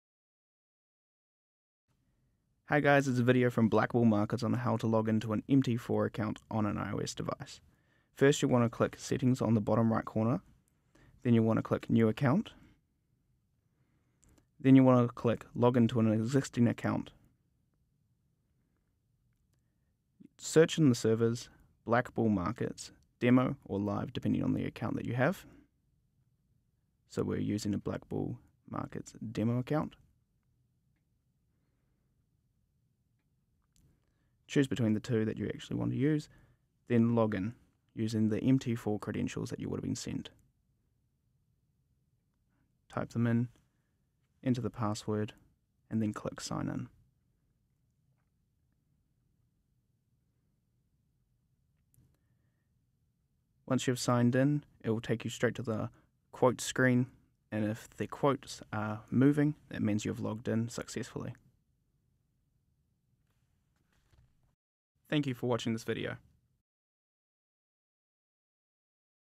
How to log in to your MT4 account on iOS - Trading support
How-to video on logging on the MT4 on iOS.

MT5 and MT4 accounts available. Ready for a live account? t.ly/kBtg

Presented by Matt Foster

Risk Warning: Derivatives are issued by Black Bull Group Limited. Losses can exceed deposits, trading Derivatives is Risky.

Trade forex on BlackBull Markets.